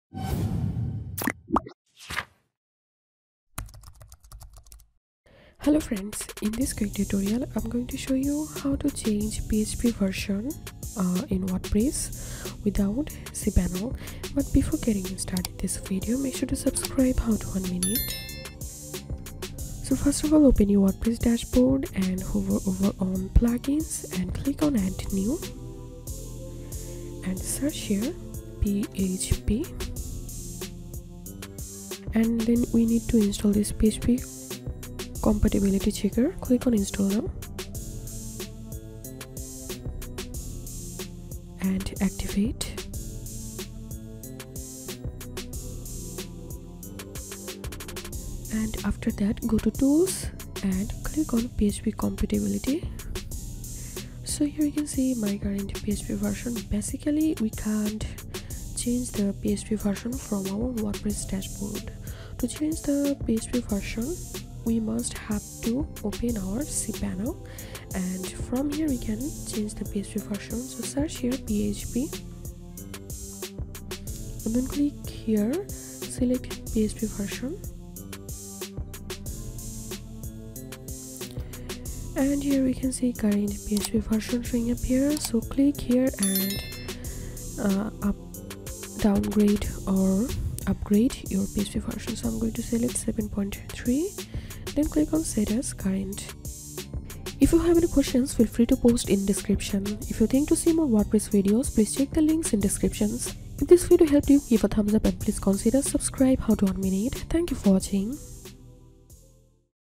How to update php version in wordpress with plugin Cpanel
How to update php version in wordpress without cpanel
Log into the control panel of your hosting provider. Under, the “Software” tab, click on Select PHP Version. On the next page, you will see the PHP version your web server is using. Now, click on the current PHP version and then select the latest version from the drop-down.